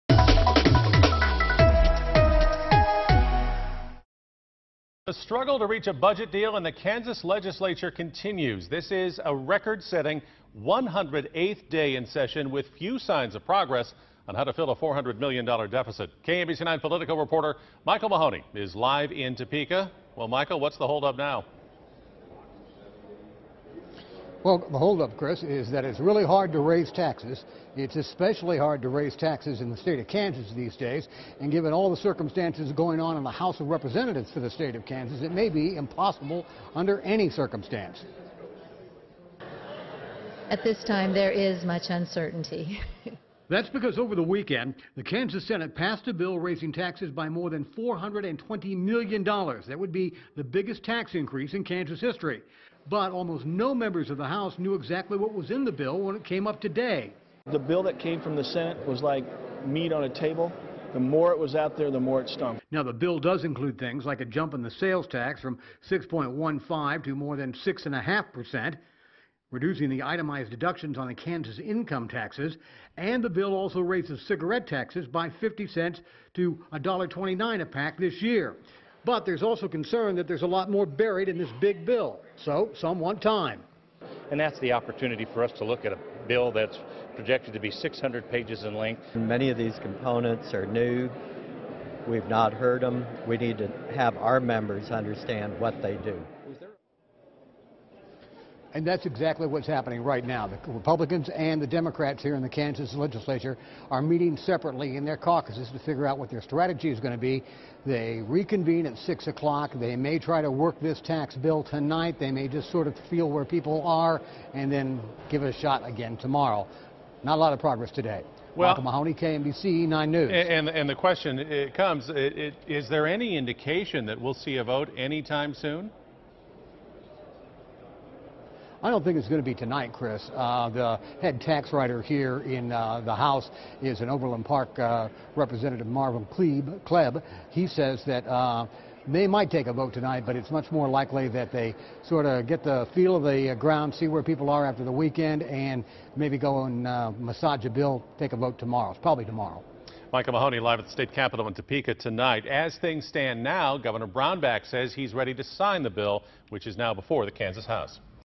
Kansas House takes time to look over tax hike plans
Kansas House members said they needed more time to look over a proposed tax increase package that was passed by the Senate over the weekend.